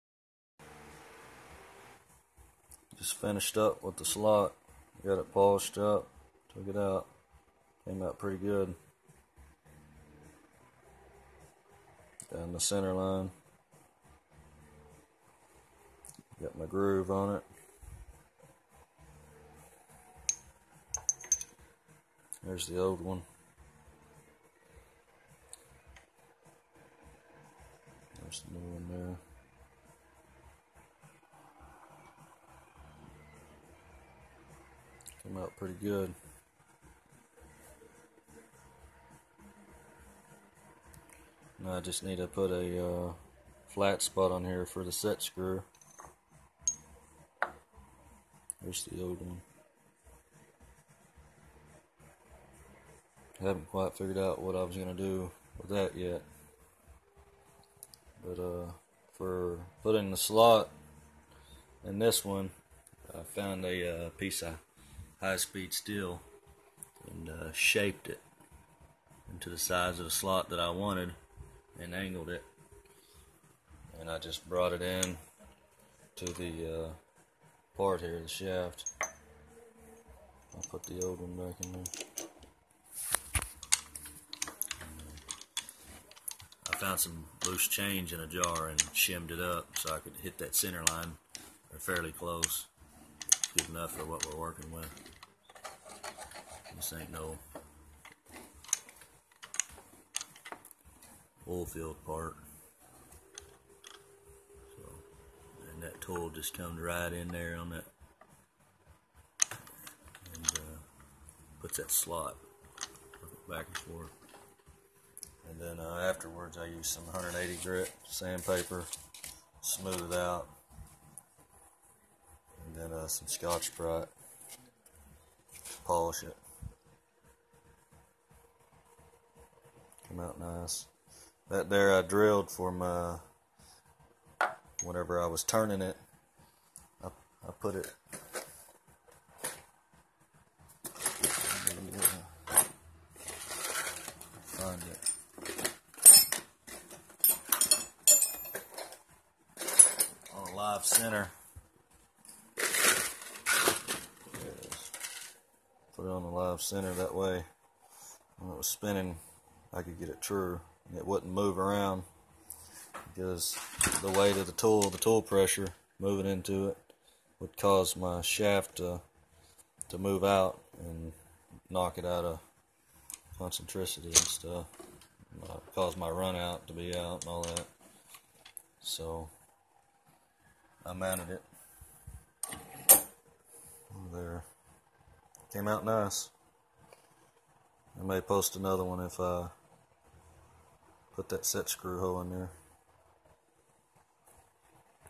Cutting a SLOT on a mini lathe
Putting a slot down the side. Just a brief demonstration of the ability of the mini lathe.